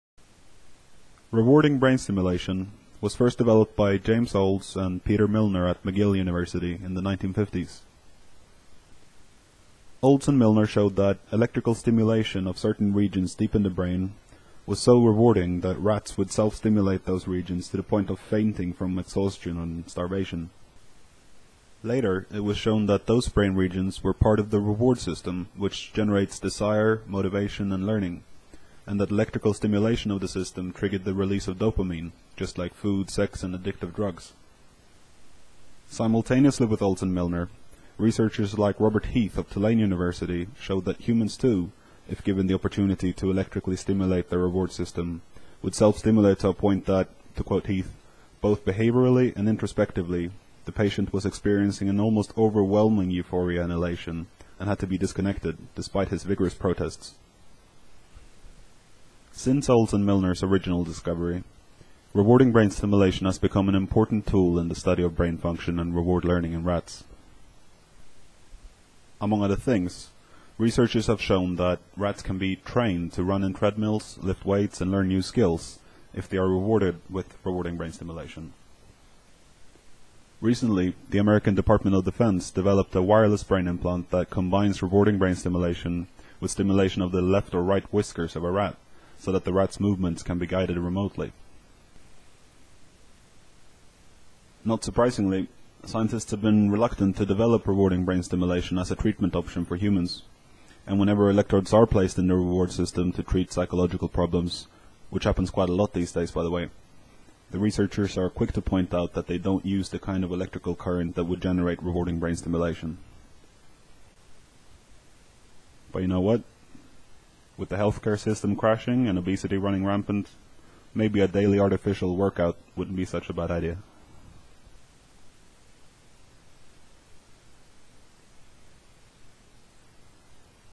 What is rewarding brain stimulation?
This video describes rewarding brain stimulation.

Images:

Intracranial self-stimulation in the rat -- © Johansen (2005) Exploring Reinforcement Processes using Intra-Cranial Self-Stimulation.

Location of medial forebrain stimulation electrodes in the rat brain -- © Bean & Roth (1991) Extracellular dopamine and neurotensin in rat prefrontal cortex in vivo: effects of median forebrain bundle stimulation frequency, stimulation pattern, and dopamine autoreceptors. J Neurosci.

Conditional rewarding brain stimulation driving weight lifting in the rat -- © Garner et al (1991) Intracranial self-stimulation motivates treadmill running. J Appl Physiol.

Conditional rewarding brain stimulation driving treadmill running in the rat -- © Burgess et al (1991) Intracranial self-stimulation motivates weight-lifting exercise in rats. J Appl Physiol.

Remote control of rat locomotion -- © Xu et al (2004) A multi-channel telemetry system for brain microstimulation in freely roaming animals. J Neurosci Methods.